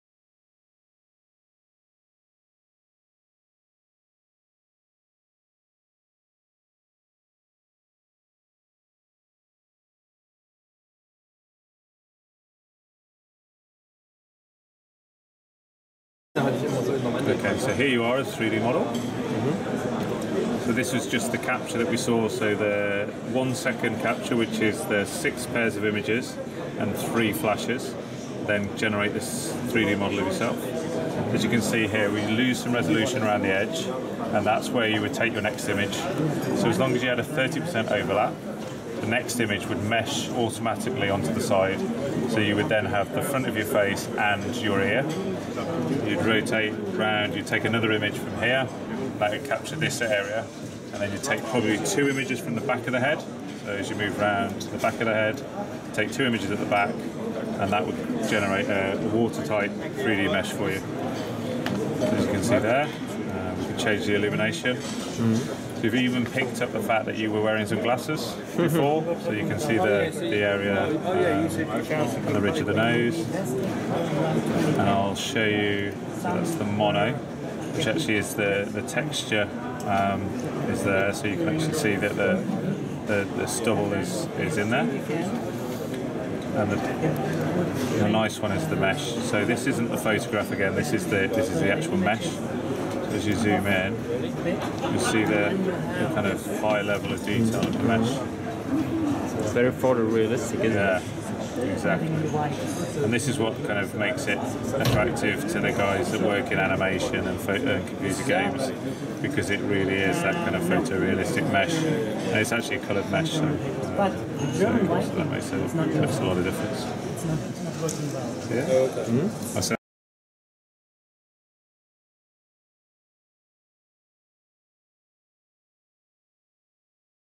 Fuel3D - a low cost high quality 3D-scanner - cubuto.com
Presentation of the Fuel3D scanner at the Inside 3D Printing 2014 in Berlin.  A high quality 3D-scanner for 3D-printing, animation or medical usage.